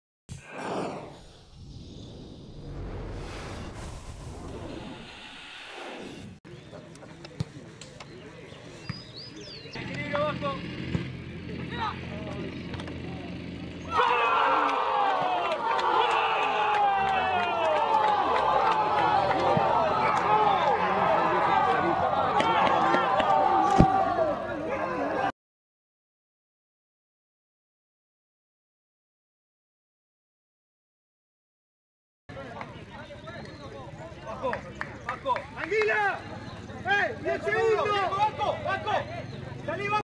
Gol de Bautista Kociubinski ante Central Córdoba de Santiago del Estero.
El mediocampista anotó en la victoria de la Reserva de Estudiantes 1 a 0 por la 6ta fecha de la Copa de la Liga. (19/03/2021) . Vídeo. Espn Argentina.